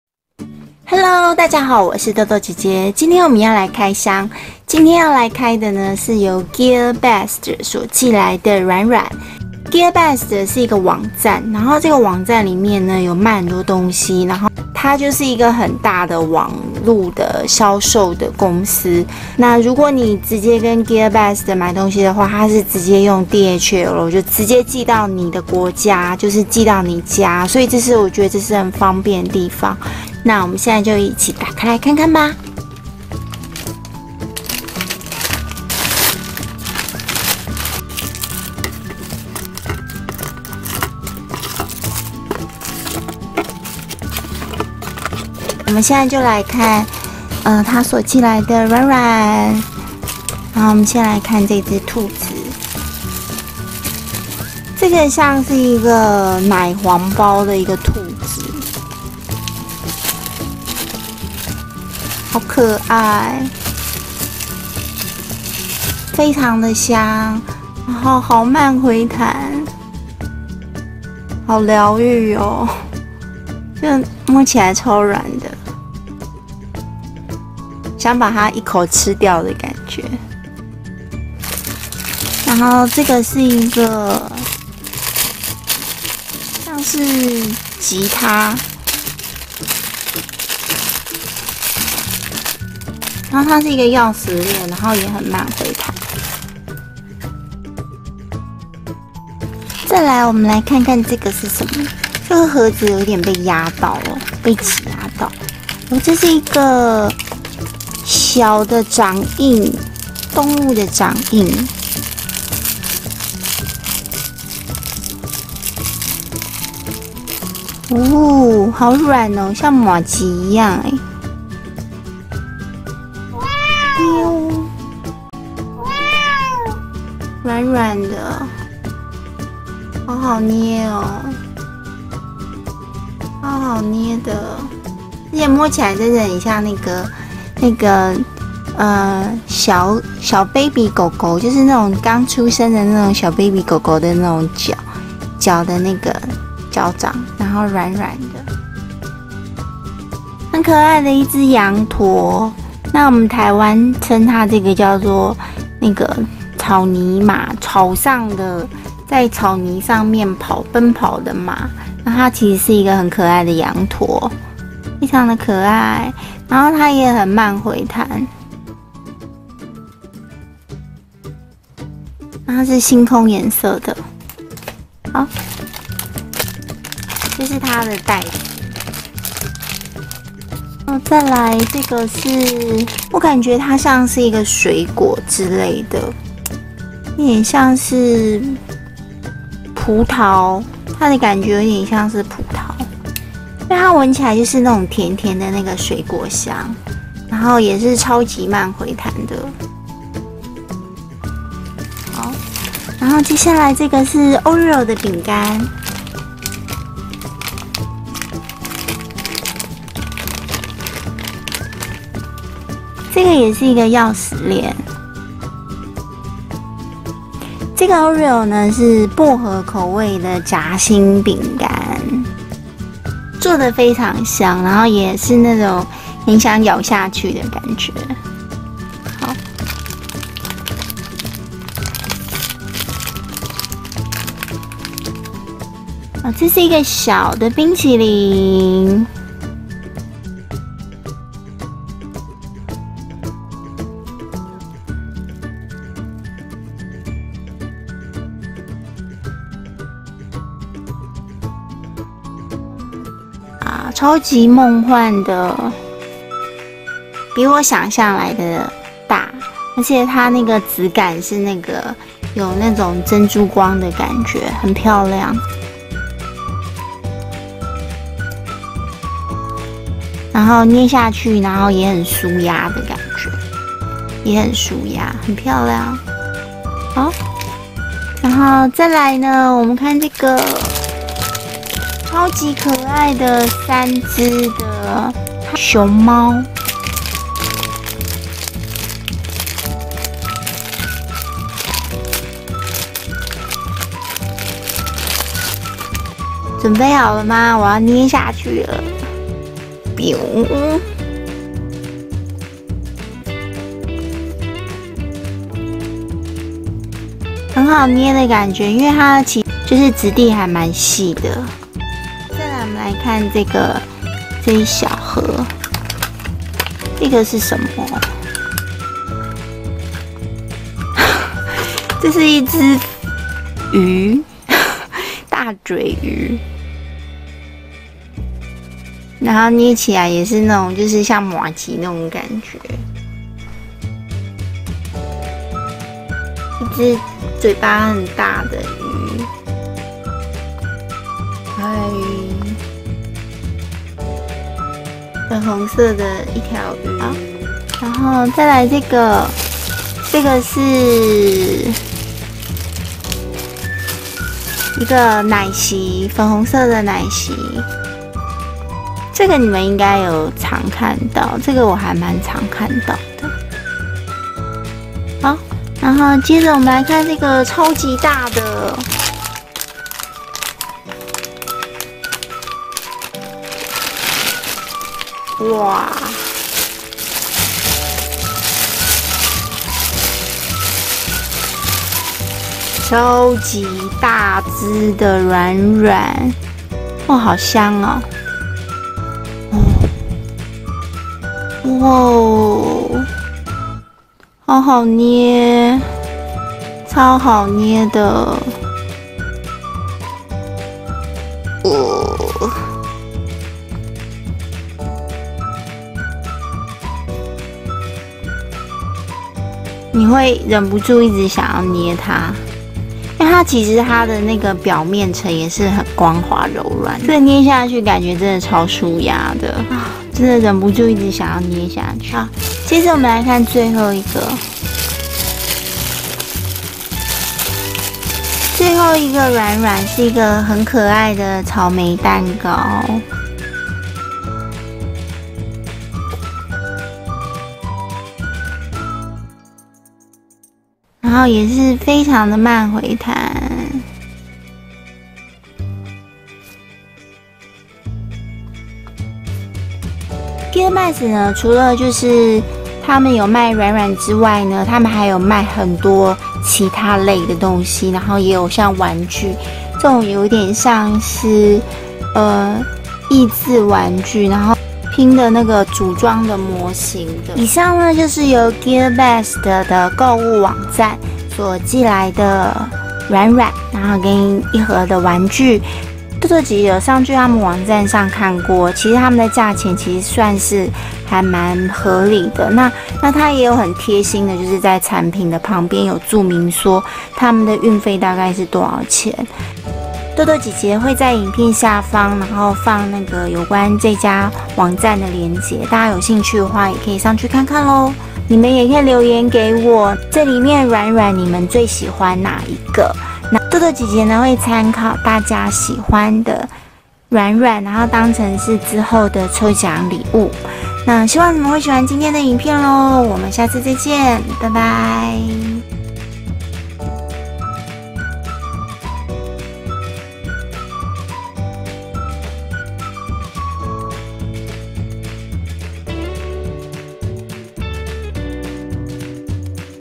GearBest 海外軟軟 開箱 史上最大軟軟動物 Huge Squishy Package
GearBest 海外軟軟 開箱  史上最大軟軟動物 荳荳玩樂趣 DoDo Toys
Huge Squishy Package

影片中的商品:
超大綿羊 折扣代碼: Sheep
貓熊疊疊樂 折扣代碼: Tarepanda
草尼馬軟軟(羊駝) 折扣代碼: liama1
彩色貝殼 折扣代碼: Seashell
草莓蛋糕 折扣代碼: StrawberryCake
DIY 手作小熊的家 折扣代碼 : BirthdayGift